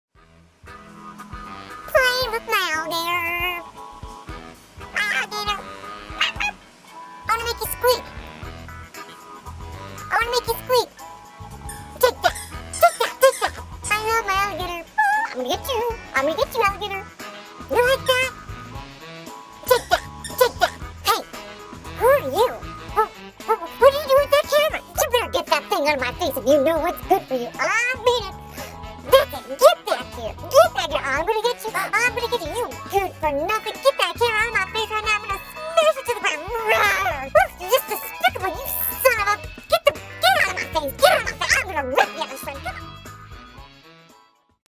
Painter - our Boston Terrier Pug (bug) gettin mad
Our 12 week old Boston Terrier Pug (bug) was getting mad at us for video taping him with my iPhone...